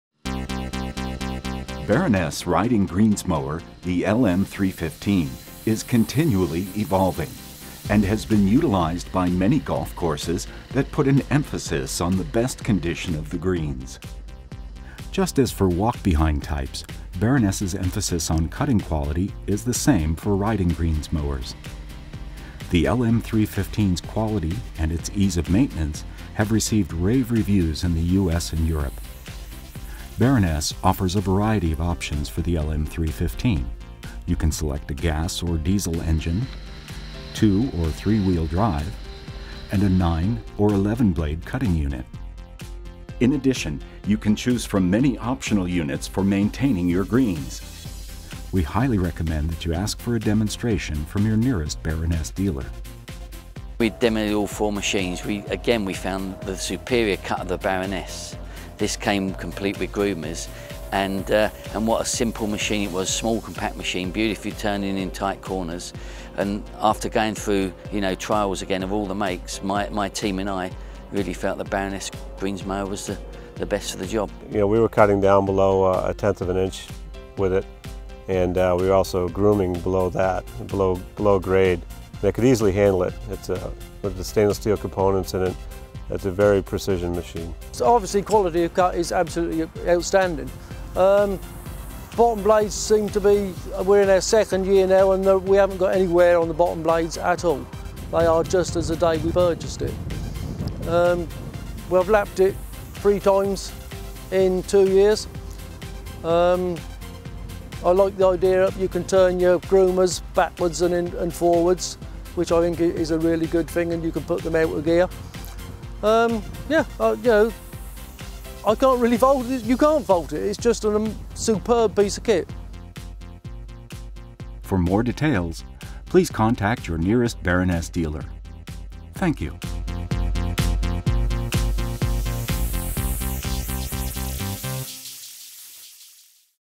LM-315 Greens- und Teesmäher
Der LM315 ist mit einem 2-Rad Antrieb, optional mit 3-Rad Antrieb ausgestattet. Die separate Aufhängung der Grasfangkörbe ga-rantiert, dass die Schneideinheiten belastungsfrei über das Grün laufen und ein sauberes Schnittbild ermöglichen. Der verstellbare Lenkholm gewährleistet eine komfortable Sitzposition.

Die schnell demontierbaren Spindeleinheiten und die leicht ver-ständlichen Bedienelemente sorgen für die von Baroness gewohn-te einfache Handhabung.